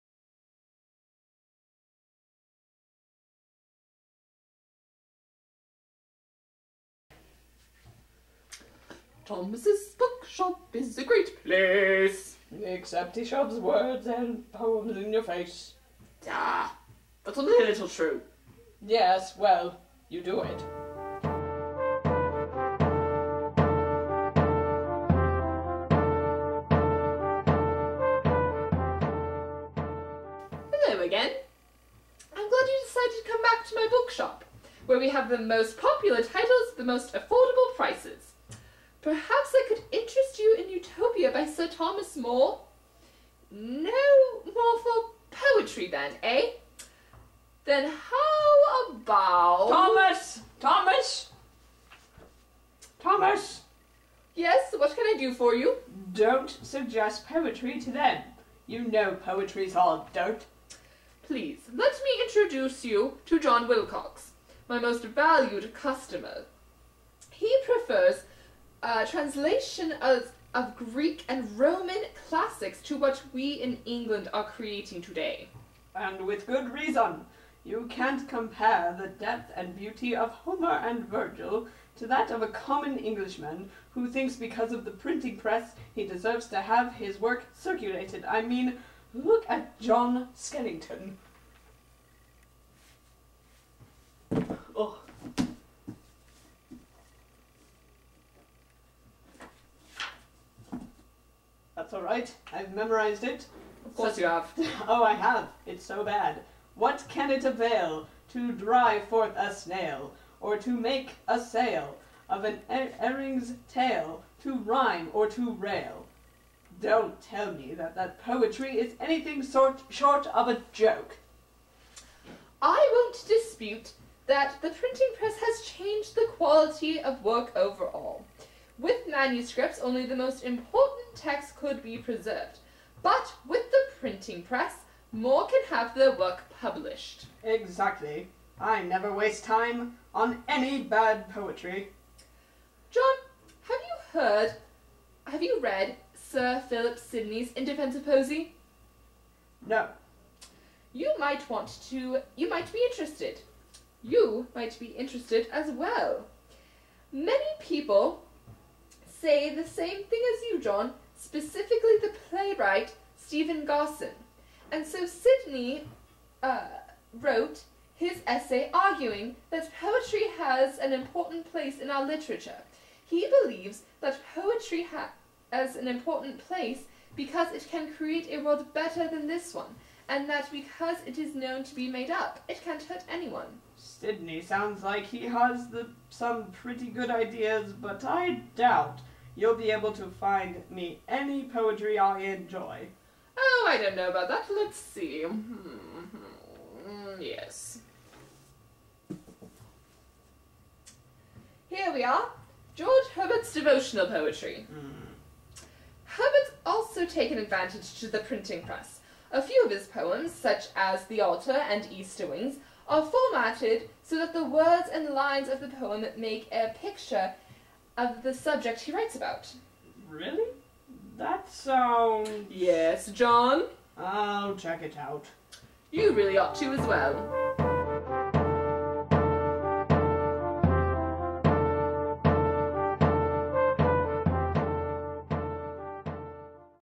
Typographia Chapter One
An explanation from Thomas and John about the printed word as it started in the Renaissance. What kind of effects did it have on authors and their texts? Did it change the quality of written work? Watch and learn.